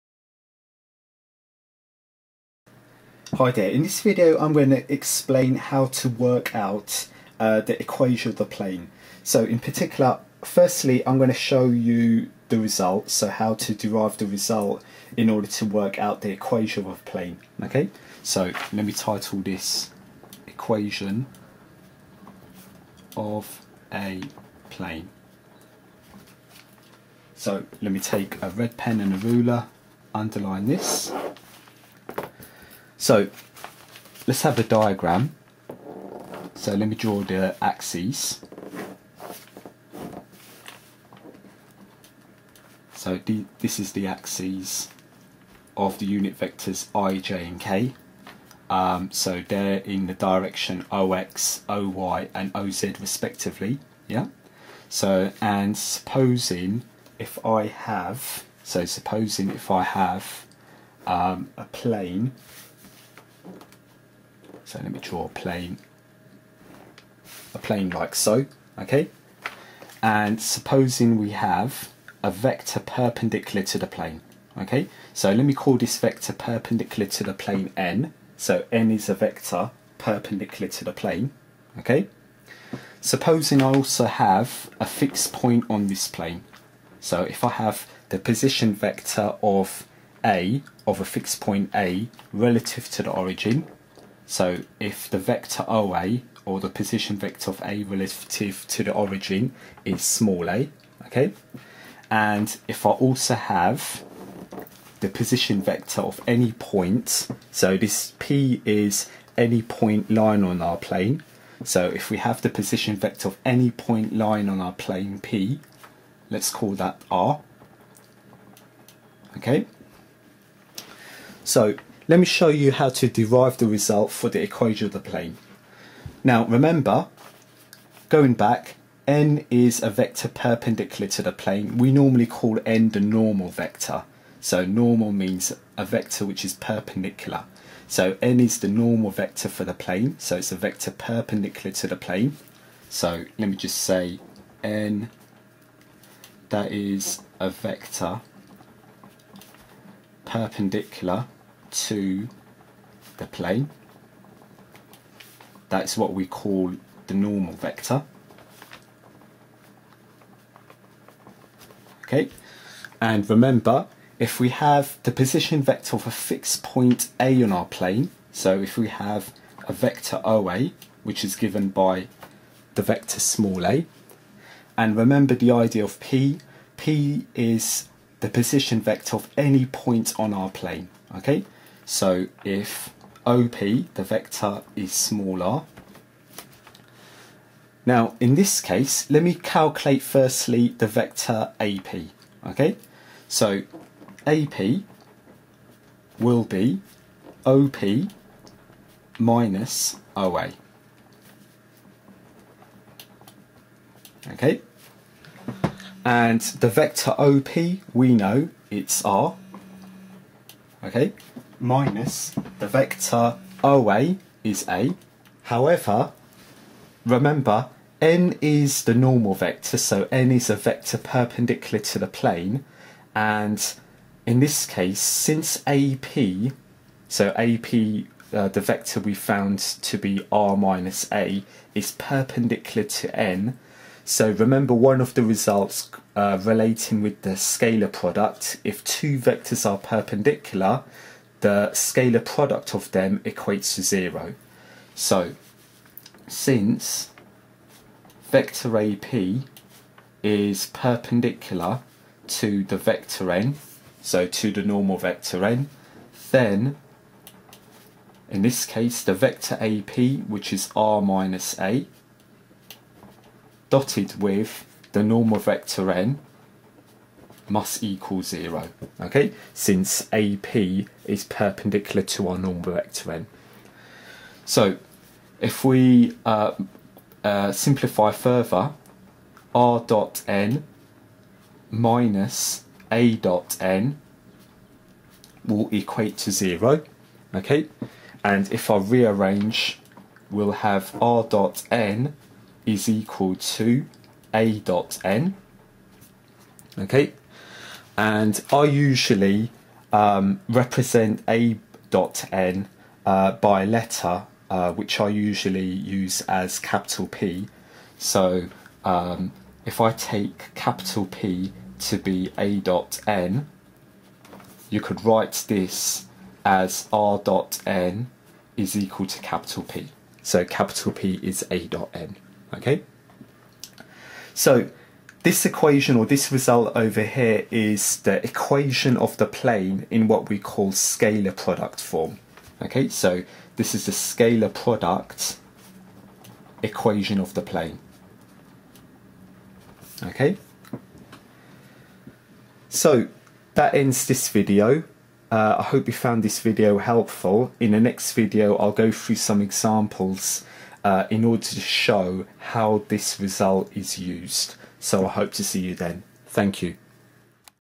AS/A-Level Maths Vectors - Equation of a plane PART 1
This AS/A-Level Maths video tutorial explains the derivation of the Scalar Product form equation of the plane.